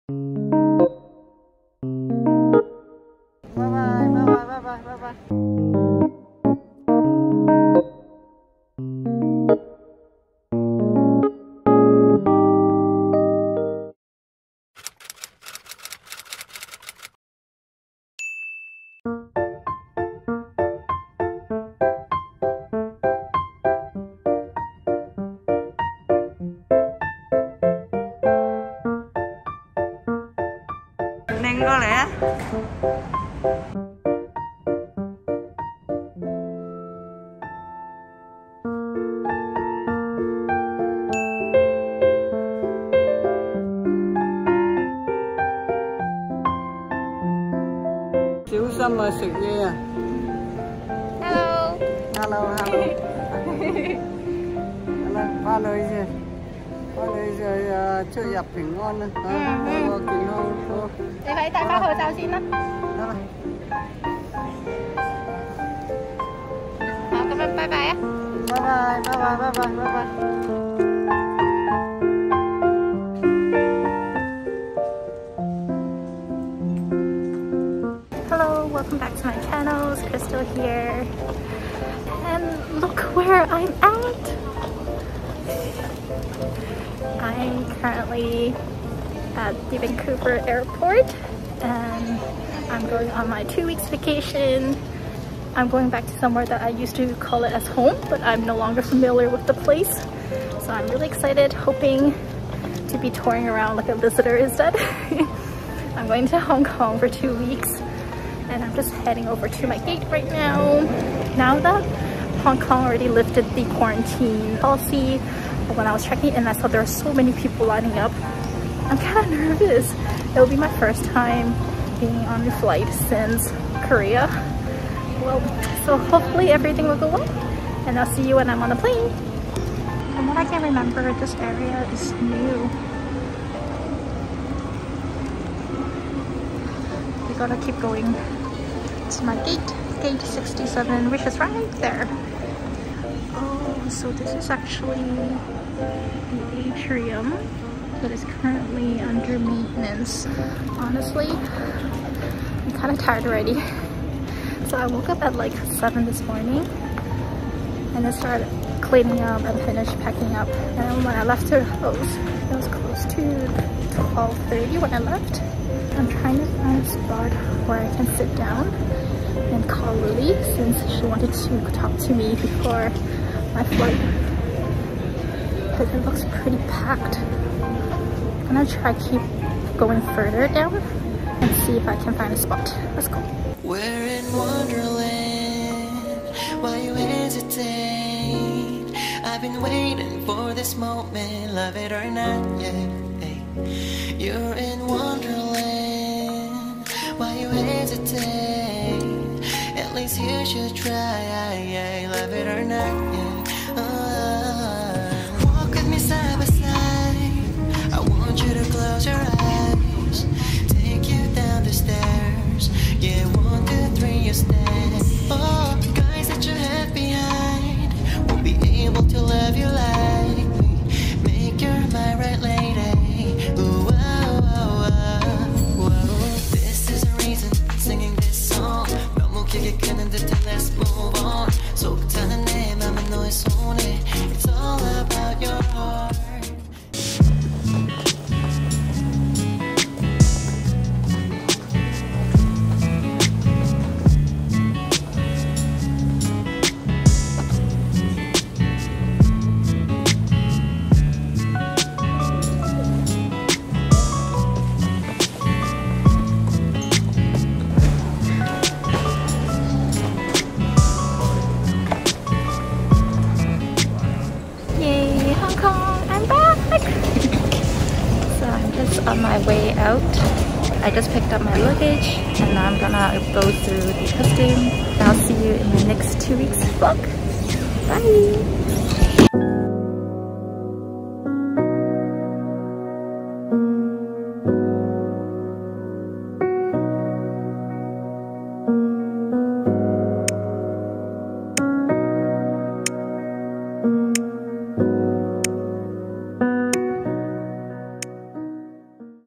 VLOG DIARIES | First flight in 3 years!! ✈️
Woohoo it's finally my turn to go on vacation! Come with me to see where I'm heading to 👀